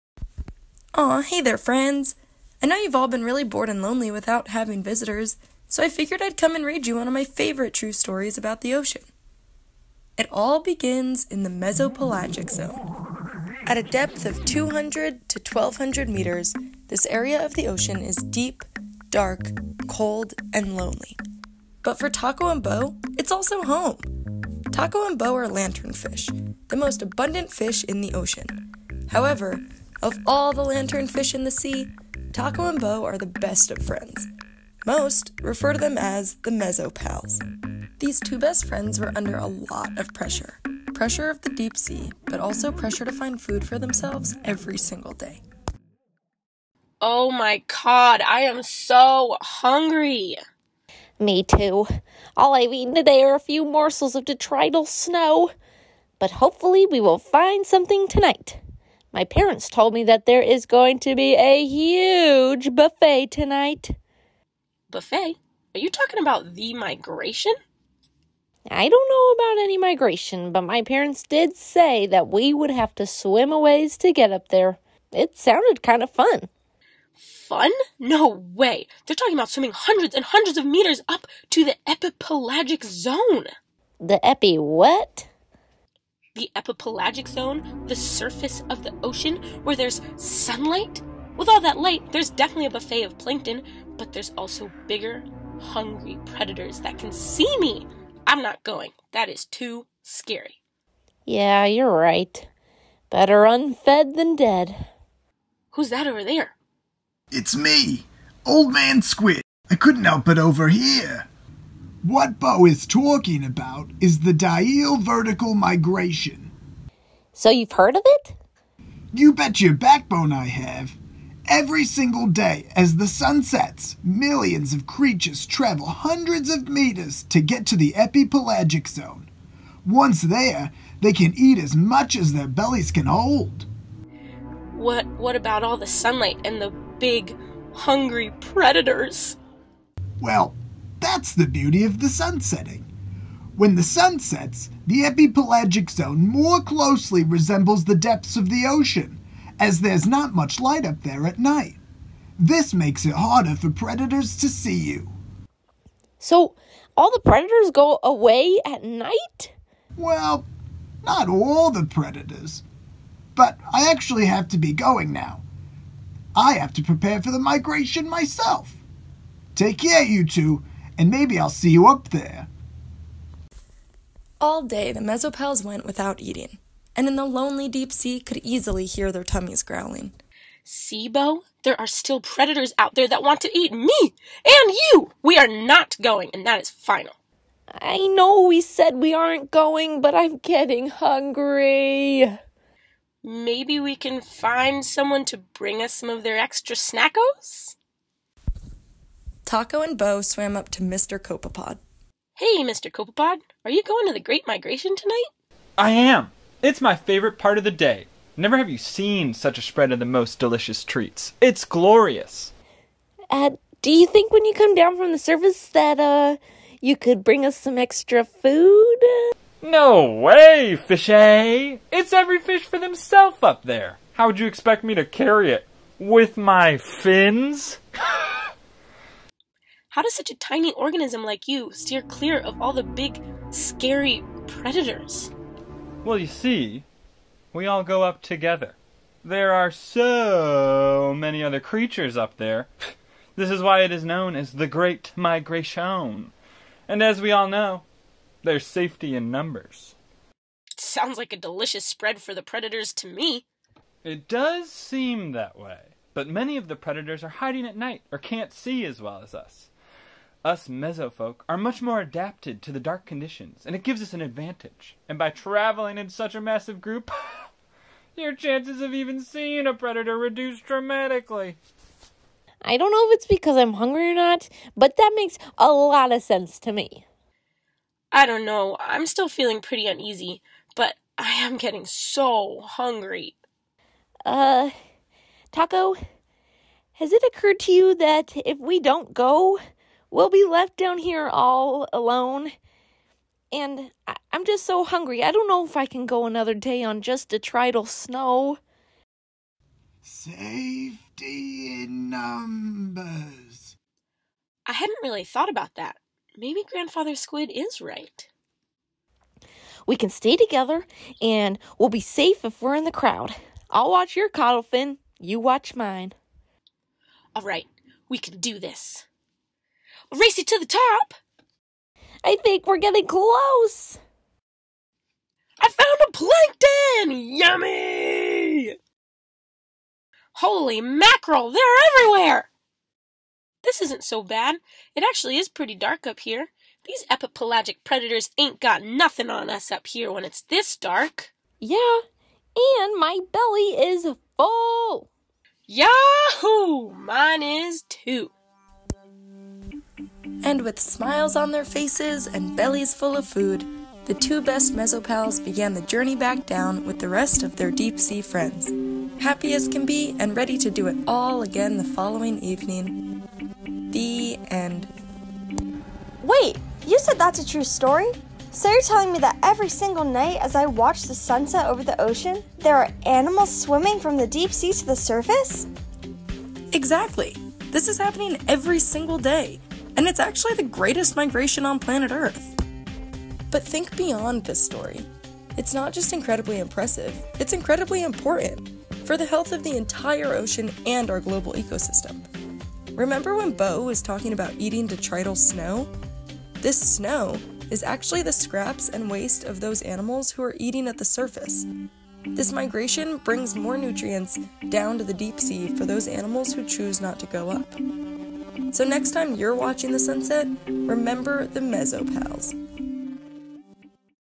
Diel Vertical Migration
Diel vertical migration is a pattern of movement used by some organisms to obtain food while avoiding predators. Watch this video from the INCREDIBLY talented Cherry Cove Staff (and a few special guests!) to learn more!